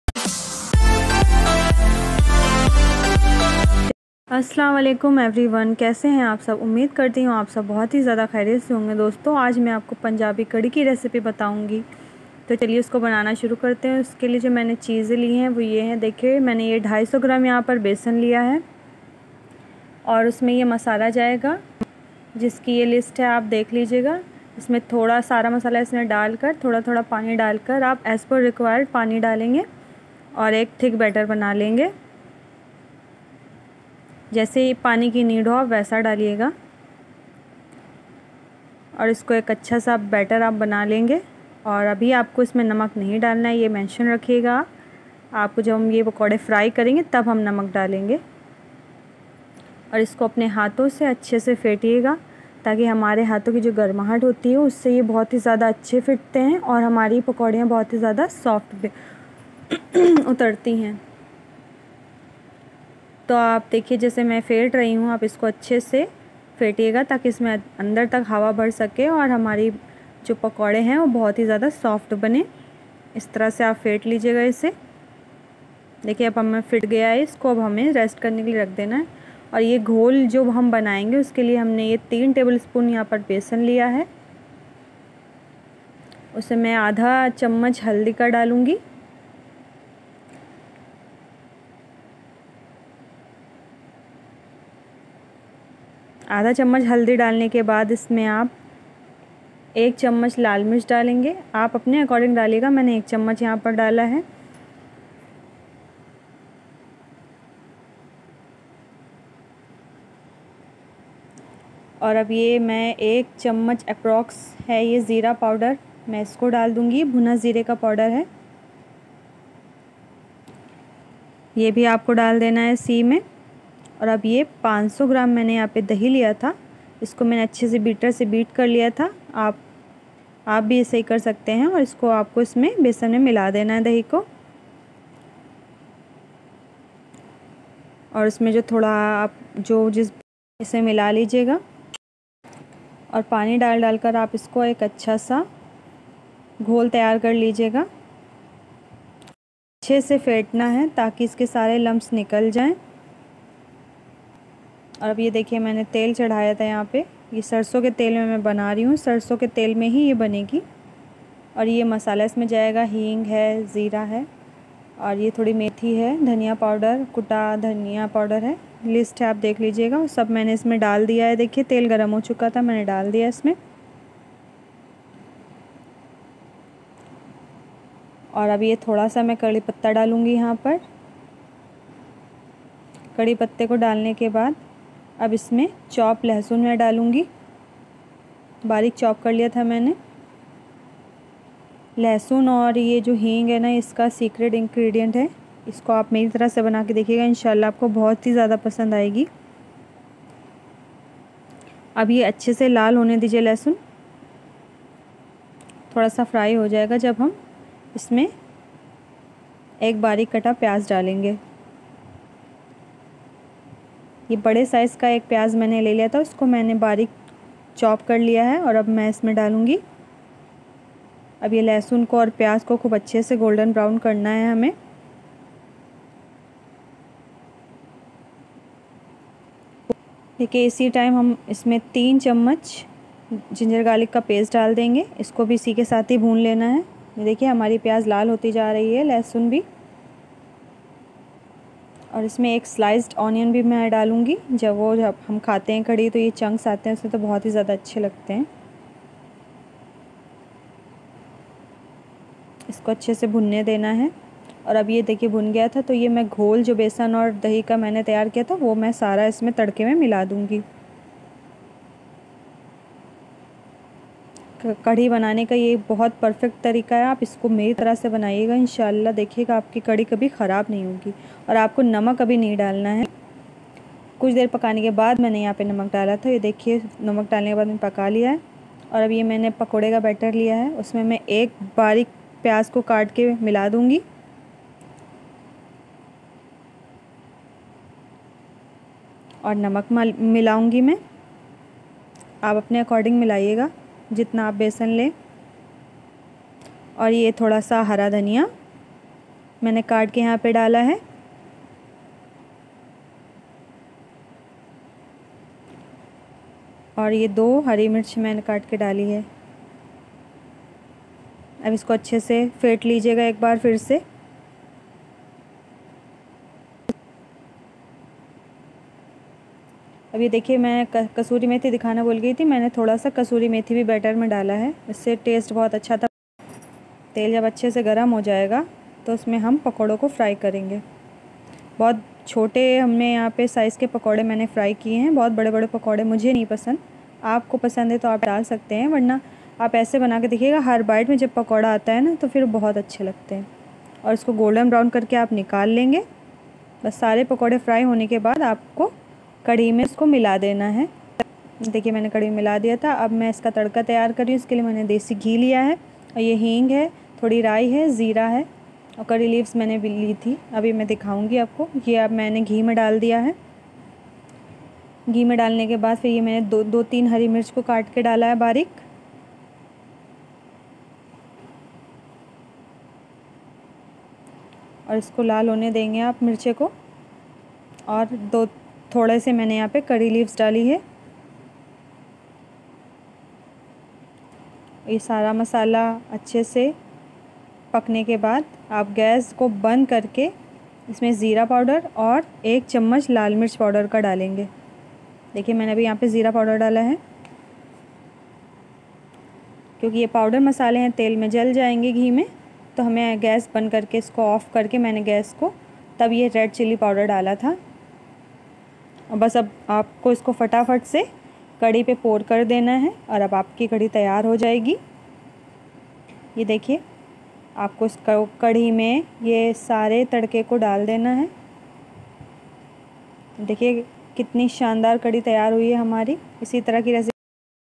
पंजाबी कढी बनाने का ऐसा तरीका अगर पहले नहीं देखा तो ये ज़रूर देखे
पंजाबी कढी बनाने का ऐसा तरीका अगर पहले नहीं देखा तो ये ज़रूर देखेpunjabikadhi
 kadhi#kadhirecipe

besan 250 gram
salt
onion chopped 1
coriander leaves
green chilli

ingredients for ghol
besan 3 tbs
dahi 500 gram

ingredients for masala
sarso mustard oil
onion 1 big size chopped finely
jeera 1 TSP
hing asofotida 1tsp
methi fenogreek 1 TSP
Kuta dhaniya 1tsp
curry leaves
garlic finely chopped

ingredients for tadka
hing1tsp
Rai mustard seeds 1 TSP  green chill 3
curry leaves